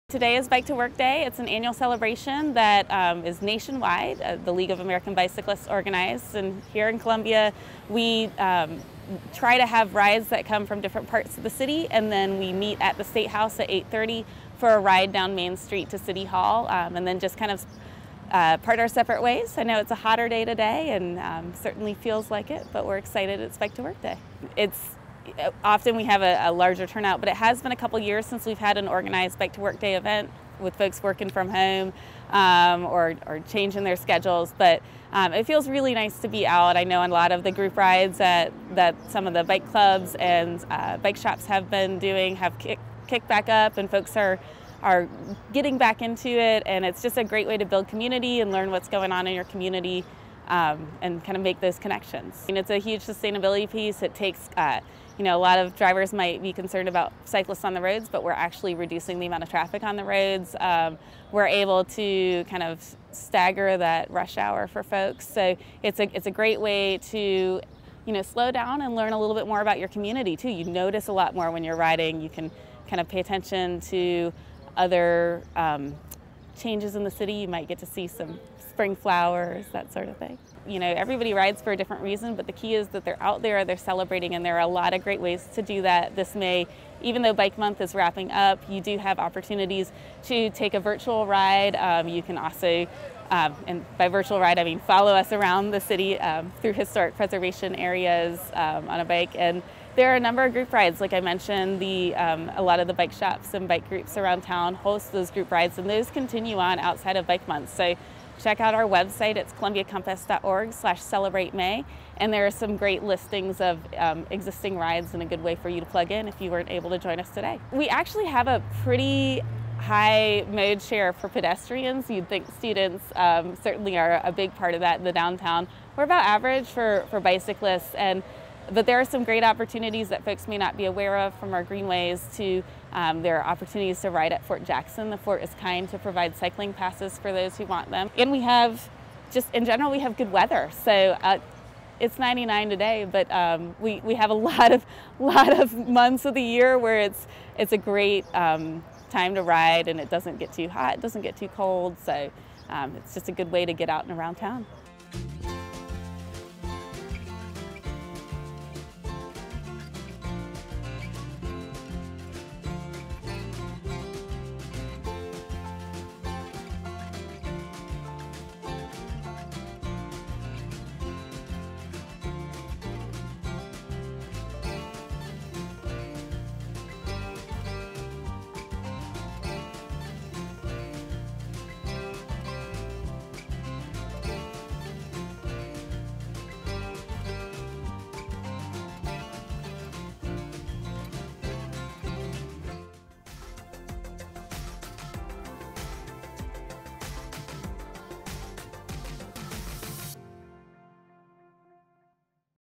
Bike to Work Day
City of Columbia employees rode from the SC Statehouse to City Hall on May 20, 2022 at 8:30 a.m. to celebrate Bike to Work Day.